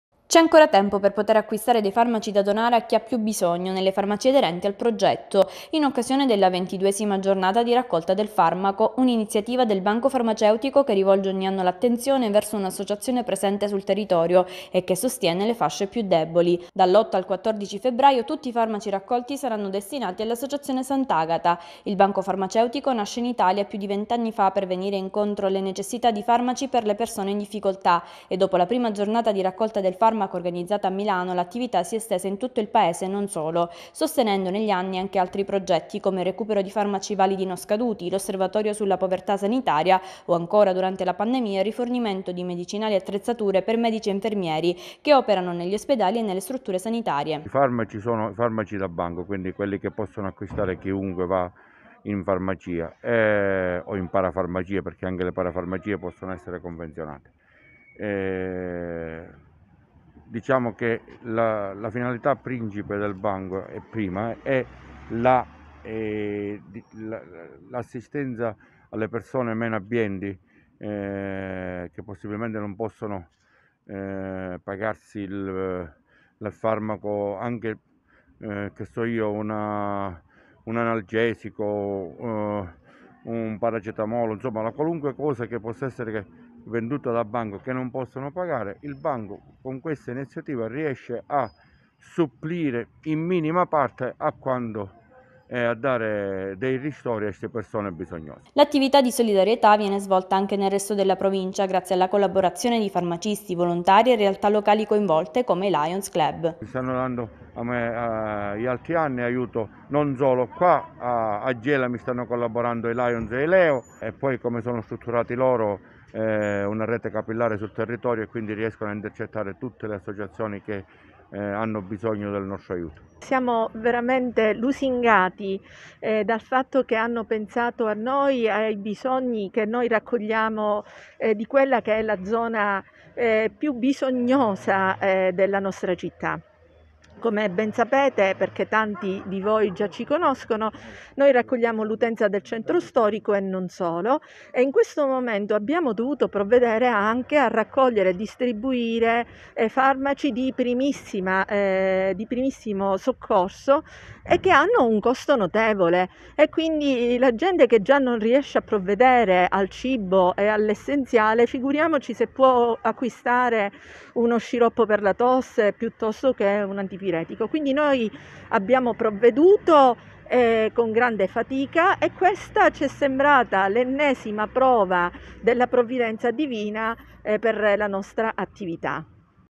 Caltanissetta - Giornata per la raccolta del farmaco - TFNWEB.IT
L’iniziativa è del banco farmaceutico in collaborazione con associazioni e club service. Fino al 14 sarà possibile donare un farmaco ai bisognosi